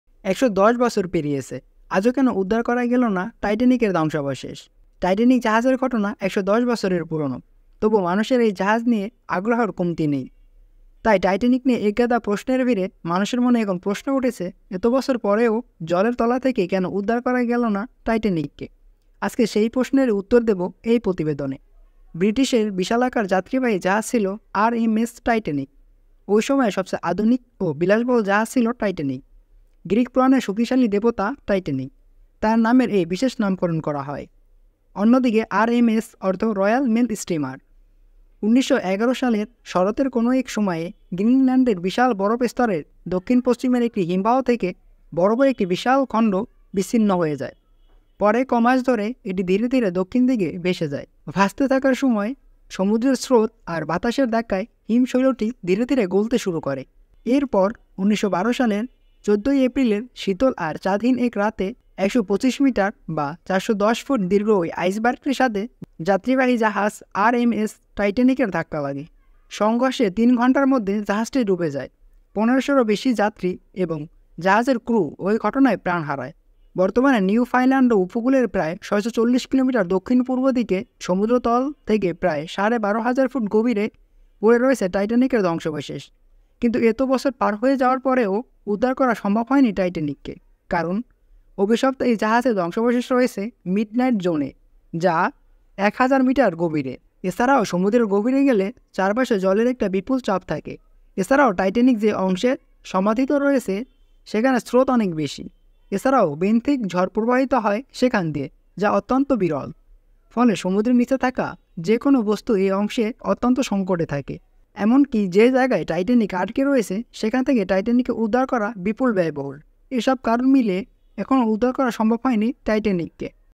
কেন টাইটানিক সমুদ্র থেকে আজও উদ্ধার করা যায়নি?.SA 10 TV
১১০ বছর পেরিয়েছে, আজও কেন উদ্ধার করা গেল না টাইটানিকের ধ্বংসাবশেষ?
আজও কেন উদ্ধার করা গেল না টাইটানিকের ধ্বংসাবশেষ? উঠে এল চাঞ্চল্যকর তথ্য   কেন টাইটানিক সমুদ্র থেকে আজও উদ্ধার করা যায়নি
টাইটানিক (Titanic) জাহাজের ঘটনা ১১০ বছরের পুরনো। তবু মানুষের এই জাহাজ নিয়ে জানার আগ্রহের কমতি নেই। তাই টাইটানিক নিয়ে একগাদা প্রশ্নের ভিড়ে মানুষের মনে এখন প্রশ্ন উঠেছে এত বছর পরেও কেন জলের তলা থেকে উদ্ধার করা গেল না টাইটানিককে। আজকে সেই প্রশ্নের উত্তর দেবো এই প্রতিবেদনে।